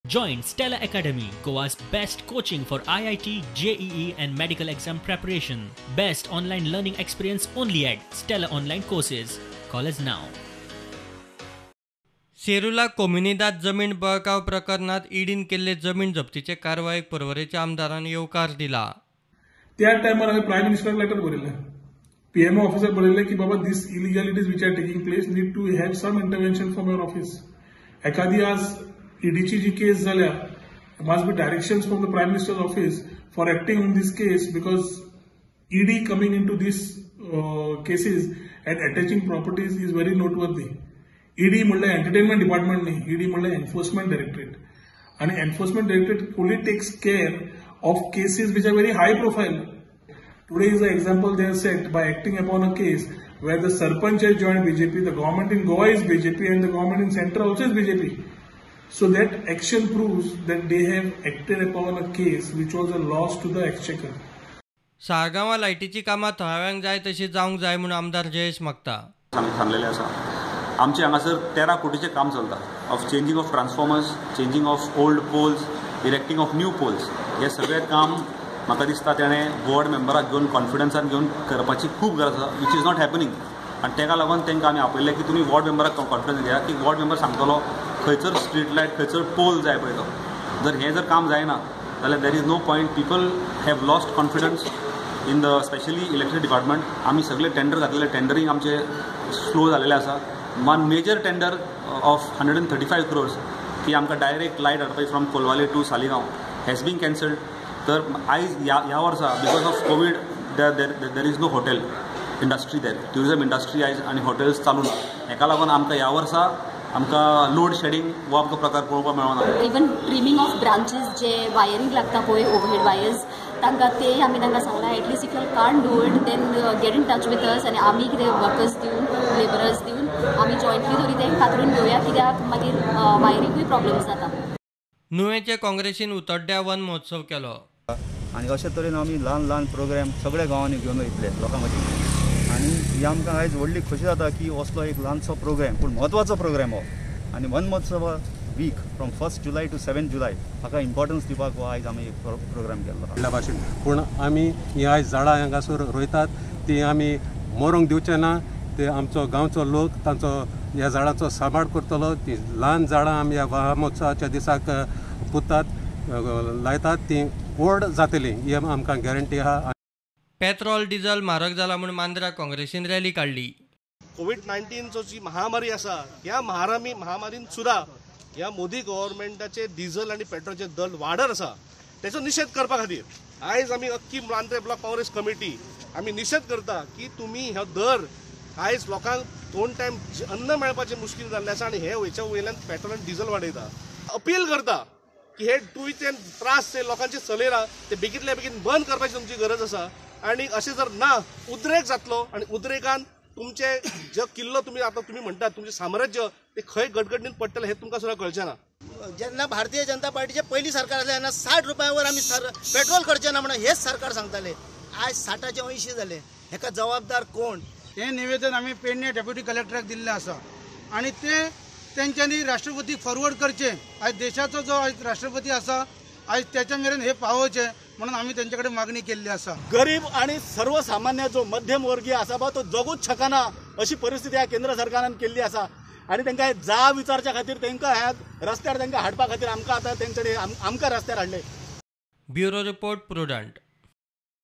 ACTION IN SERULA COMUNIDADE LAND CASE OUTCOME OF MY LETTER TO PM: KHAUNTE_Prudent Media Goa
Porvorim MLA Rohan Khaunte has informed that he had written to Prime Minister Narendra Modi over misuse of land in Goa and that ED's action in Serula Communiadade is the outcome of the letter. Saligao MLA Jayesh Salgaonkar has stated that Rs 13 Cr electricity works have been taken up in Saligao. Salgaonkar has held a meeting with the electricity department staff.
Meanwhile, Mandrem Congress Committee has held a rally protesting against unprecedented rise in fuel price.